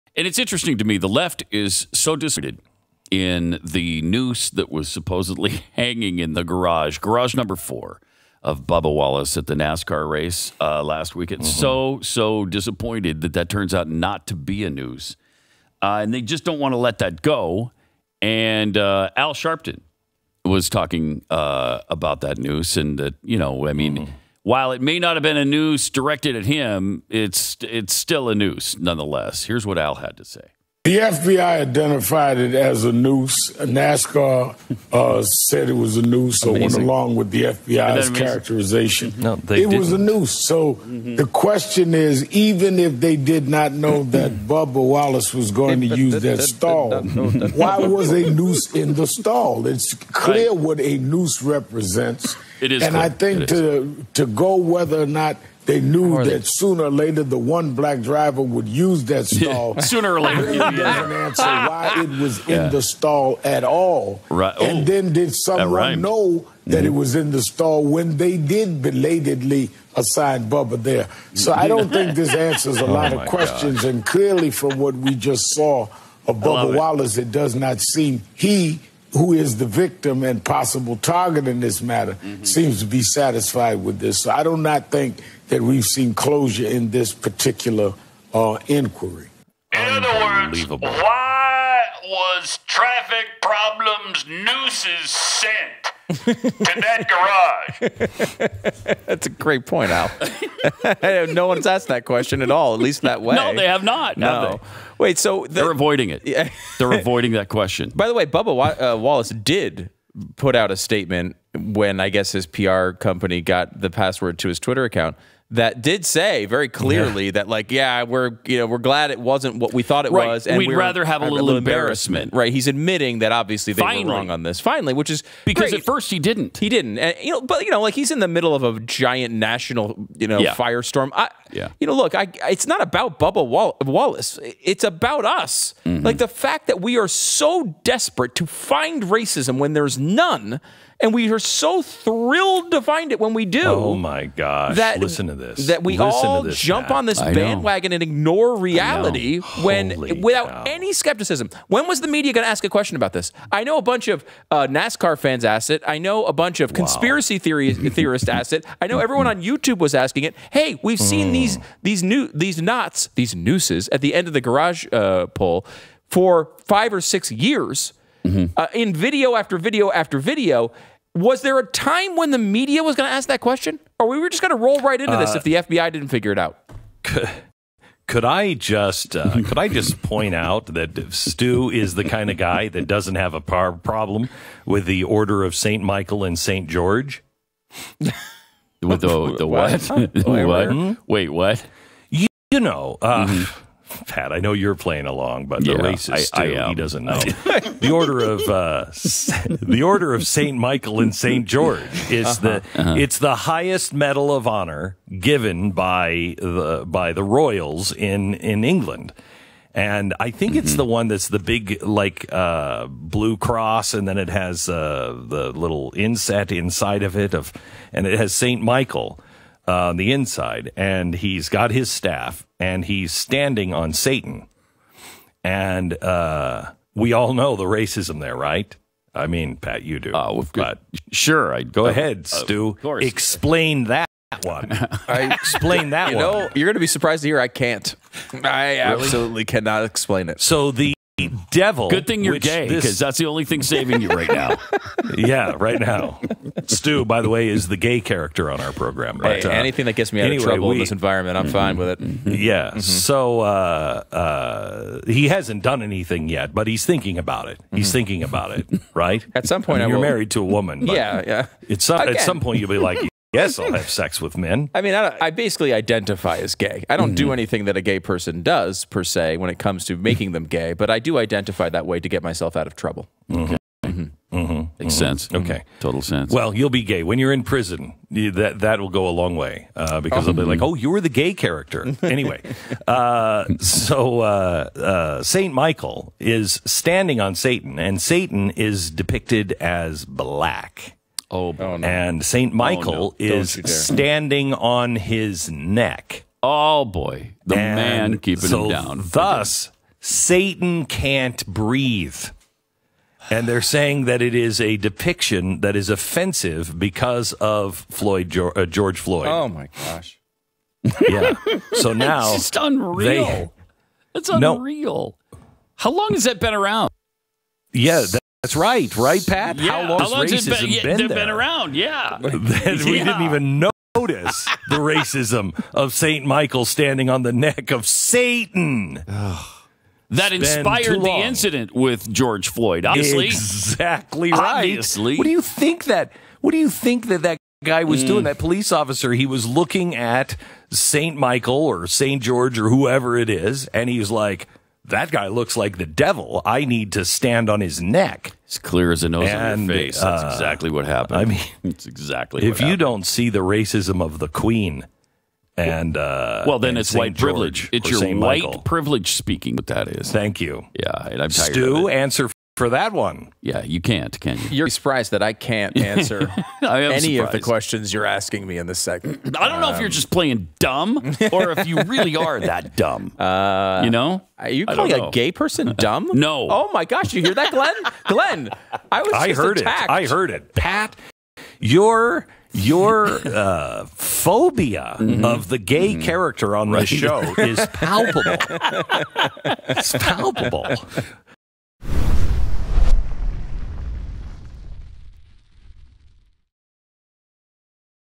DESPERATE TO FIND RACISM: Nascar 'noose' left for Bubba Wallace was… not a noose
After an investigation by the FBI and NASCAR, it was determined the noose found in the garage used by driver Bubba Wallace was...not actually a noose. Bubba Wallace said he's "embarrassed" by the incident but glad it wasn't a targeted, racial attack. So, then, WHY can't Al Sharpton let it go, too? Why are so many on the left so DESPERATE to find racism and cannot celebrate with the rest of us in situations like this one?